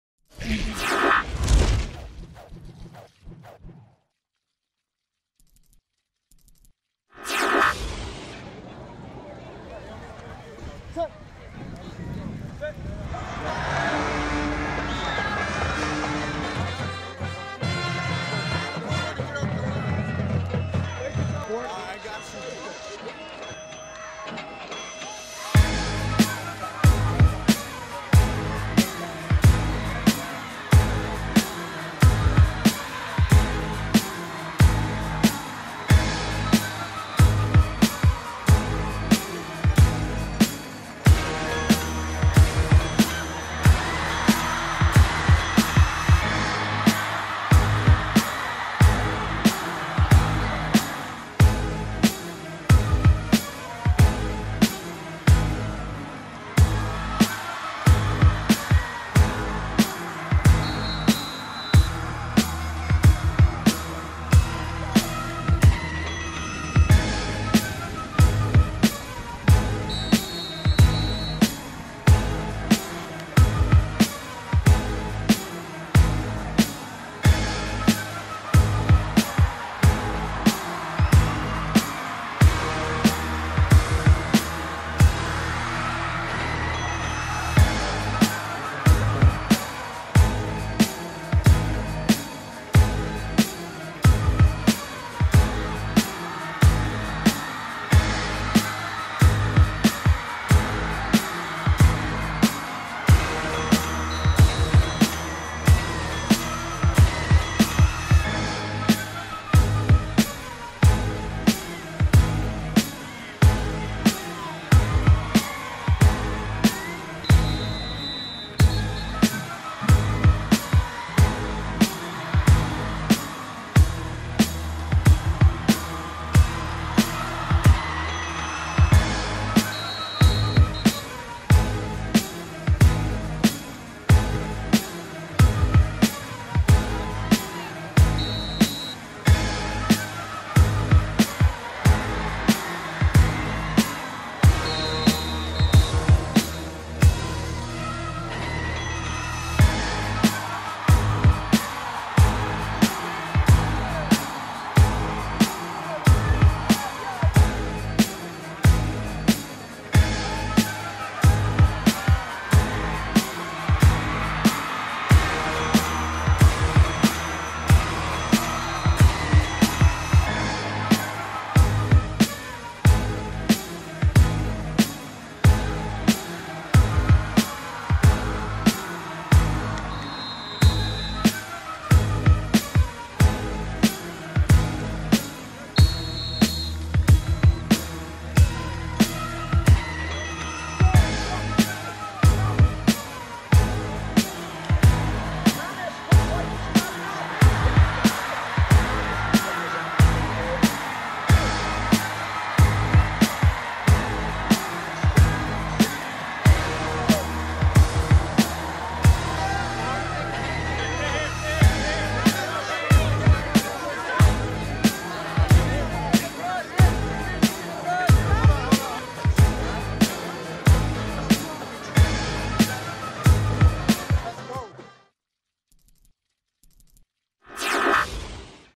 RB Zidane Thomas Senior HIGHLIGHTS!!! San Diego State signee
Zidane Thomas is a load to take down. He will do BIG THINGS at SDSU...HUGE game in AIA State Championship Game!!! National Prep Stars on PassportplayasTV